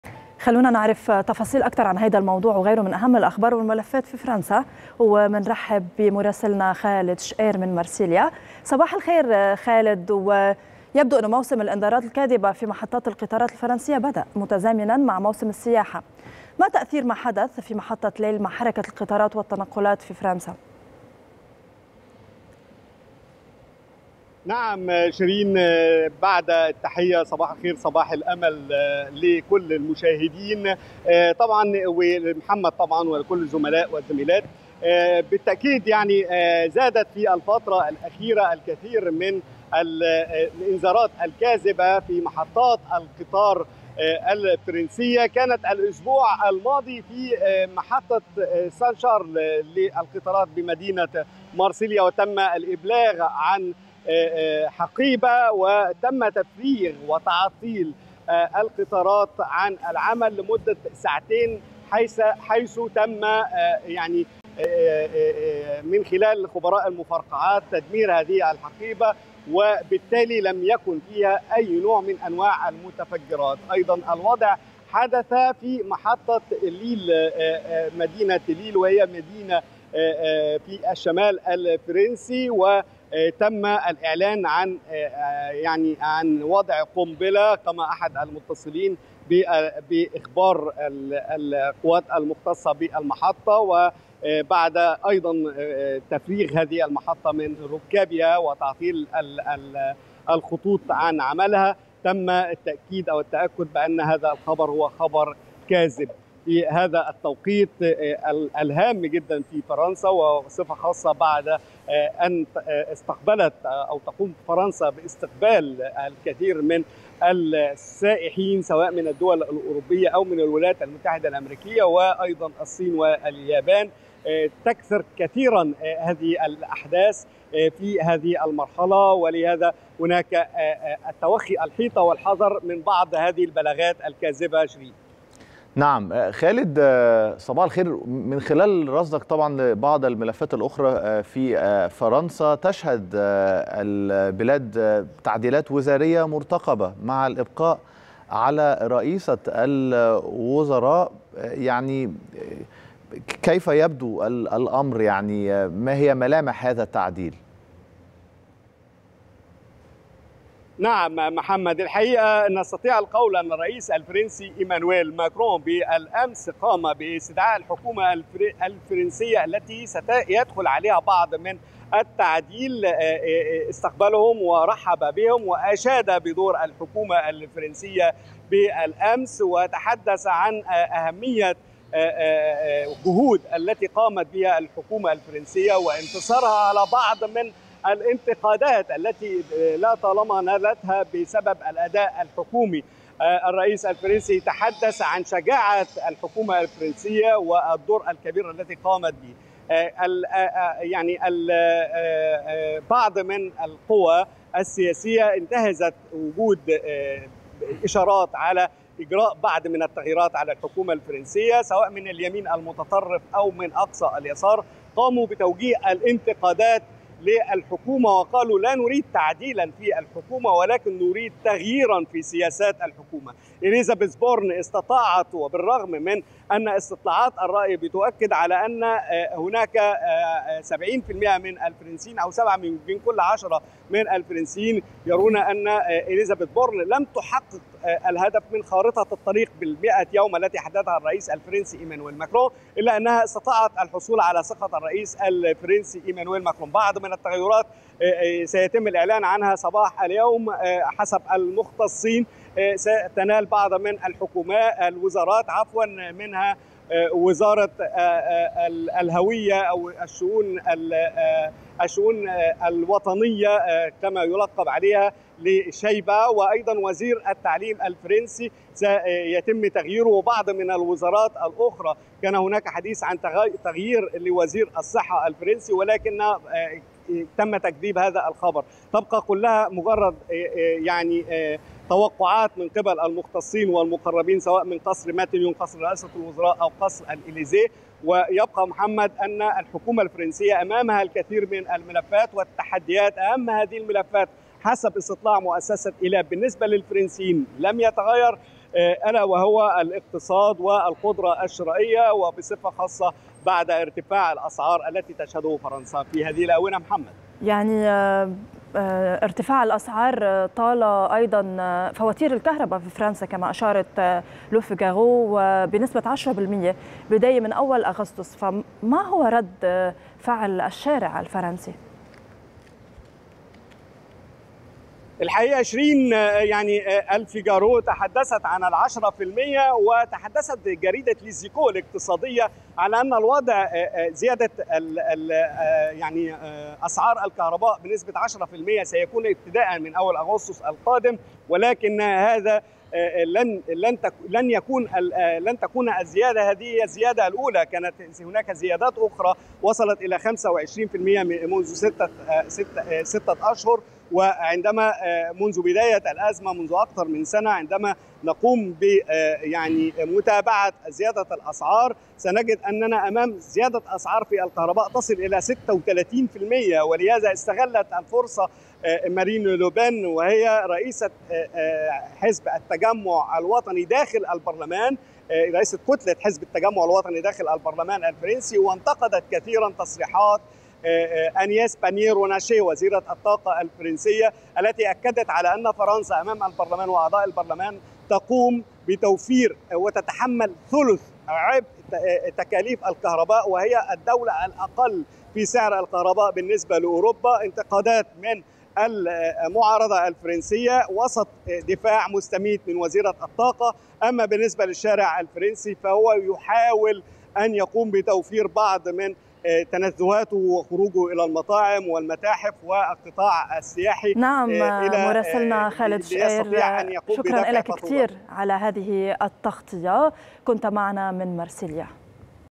إخلاء محطة ليل - أوروبا للقطارات في فرنسا بسبب إنذار كاذب.. التفاصيل مع مراسلنا
إخلاء محطة ليل - أوروبا للقطارات في فرنسا بسبب إنذار كاذب.. التفاصيل مع مراسلنا خالد شقير

20/7/2023